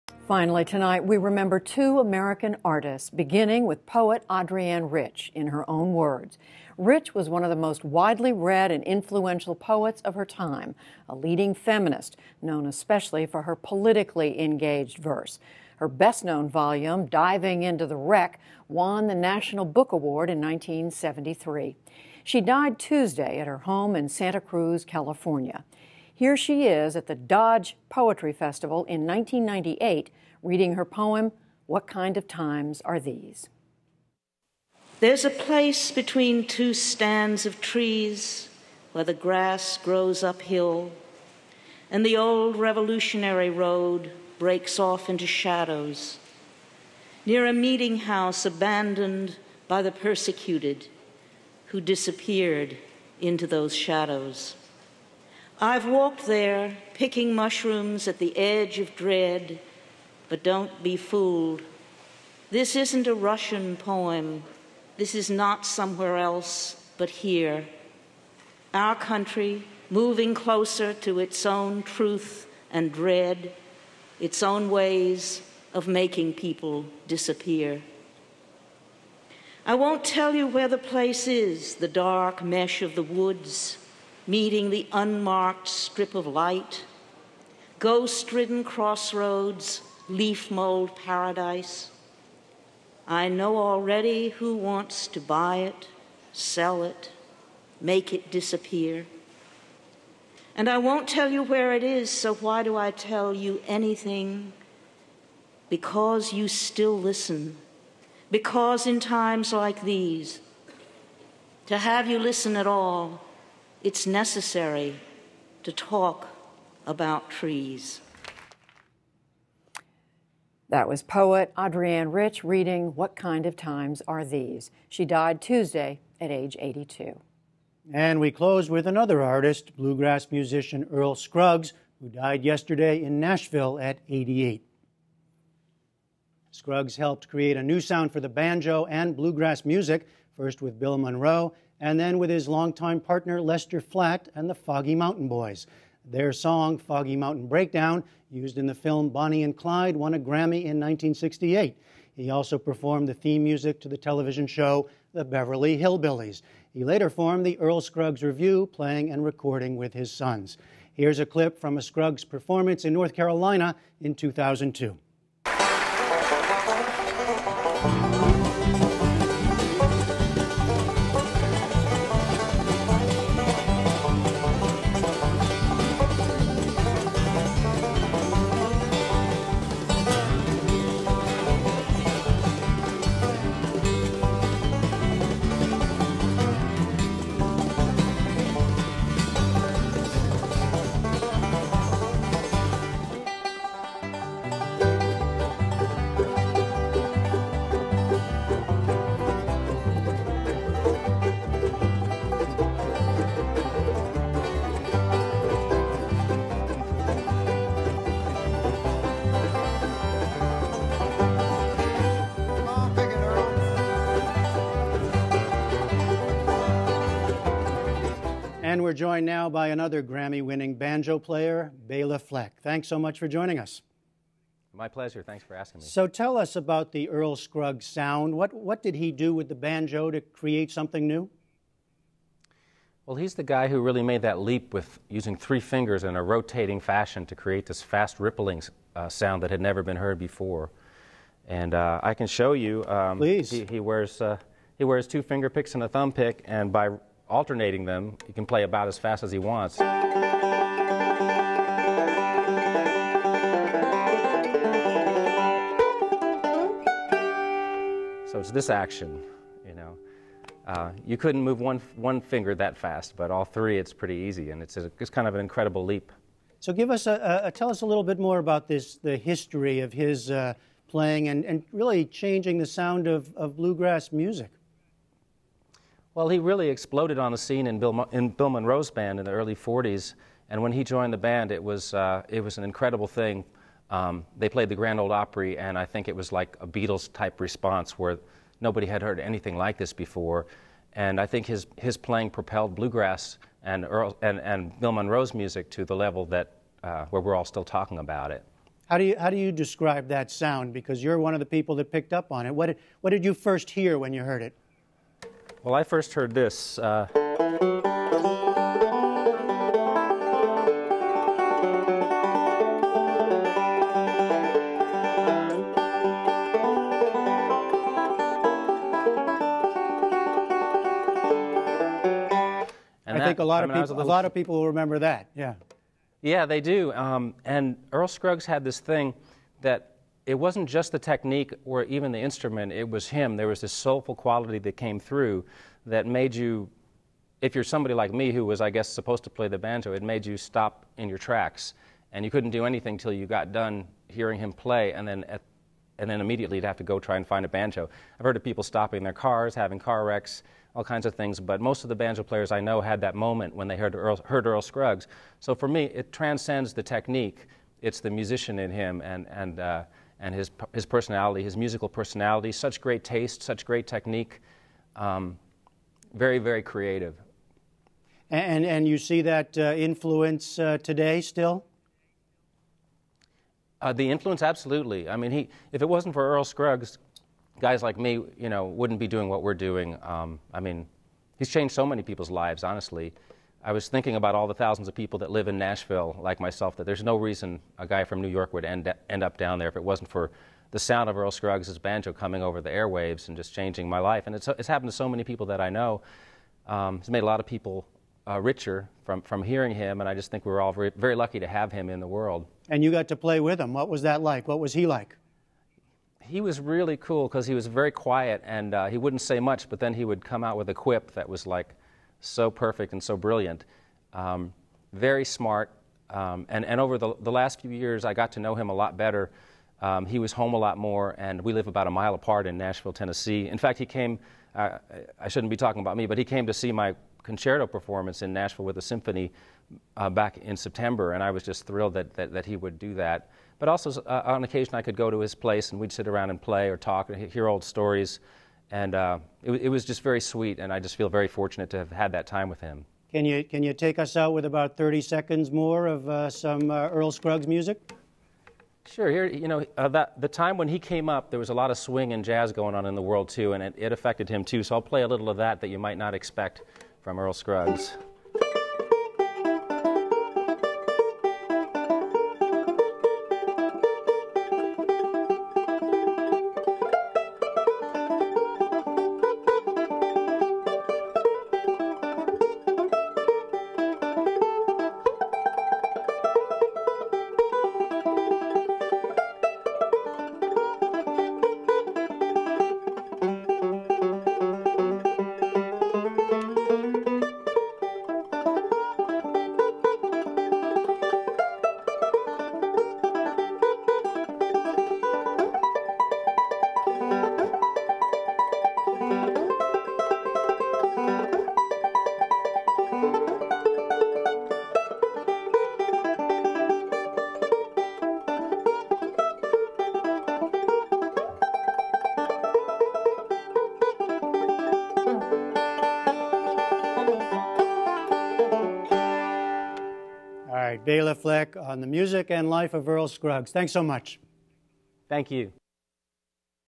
Remembering 'Brilliant' Banjo Player Earl Scruggs, Feminist Poet Adrienne Rich
Judy Woodruff reports on the death of widely read and influential poet Adrienne Rich, who died Tuesday at age 82, then Jeffrey Brown takes a look back at the life and groundbreaking sound of banjo legend and bluegrass musician Earl Scruggs with Bela Fleck, another Grammy-winning banjo player.